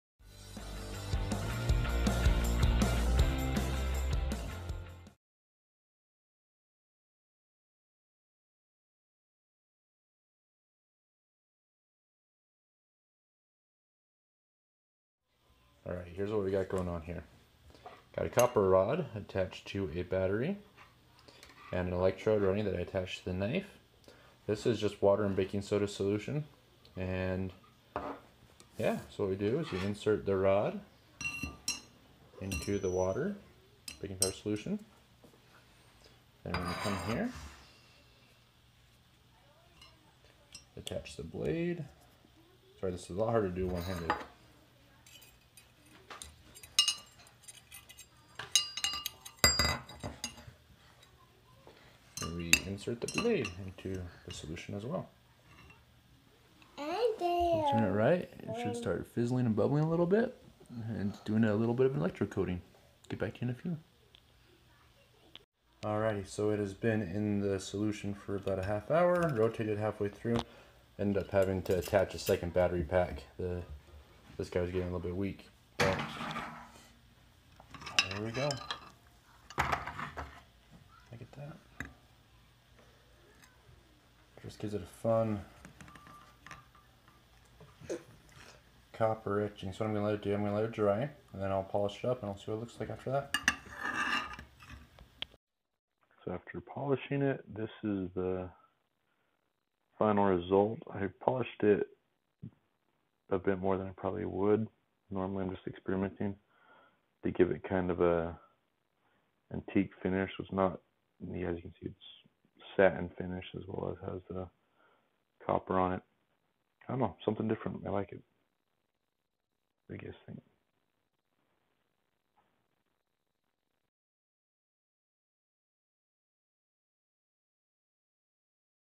Teddy Bear knives, YAY or NAY, Copper Electroplating Knife Finish
Attempting a simple diy home electroplating.

All that was needed,
Baking Soda
Water
9v battery
Copper rod
Something to plate (knife, coin, etc...)